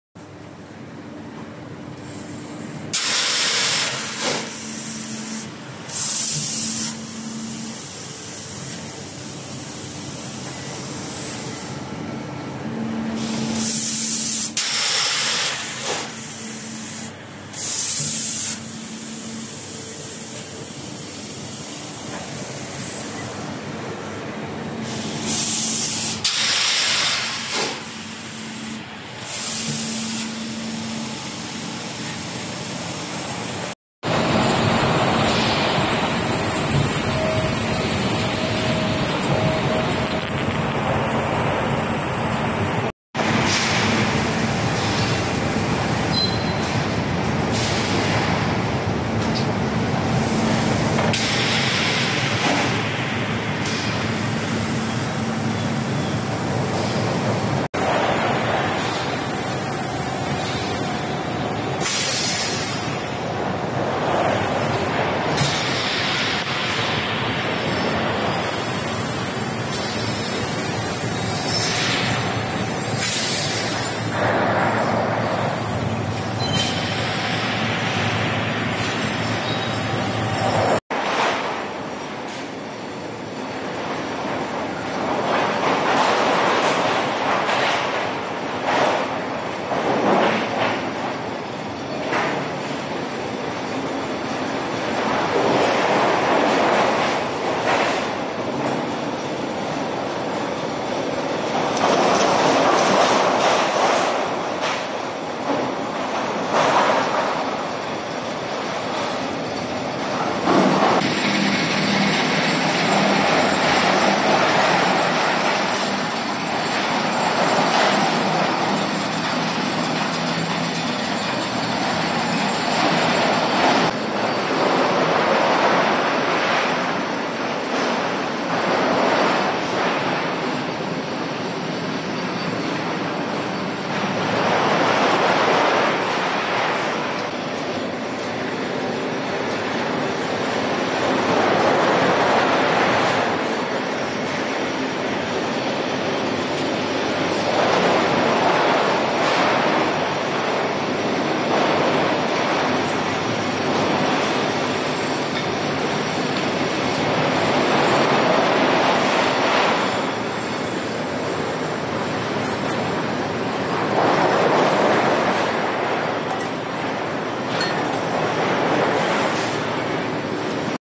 judberd casting production
Dear friends, 🌸

we are now producing castings using automated production lines. 💐

You can see that our efficiency has greatly improved compared to traditional methods across molding, molten iron spheroidization, casting, and sand removal stages. 🌺

Additionally, our use of spheroidization agents ensures stricter control over chemical elements, while mechanical molding along vertical lines results in denser structures compared to manual methods, thus leading to better surface quality for castings. 🌼

Consequently, we can offer higher quality at lower prices than before. 🌹 🌹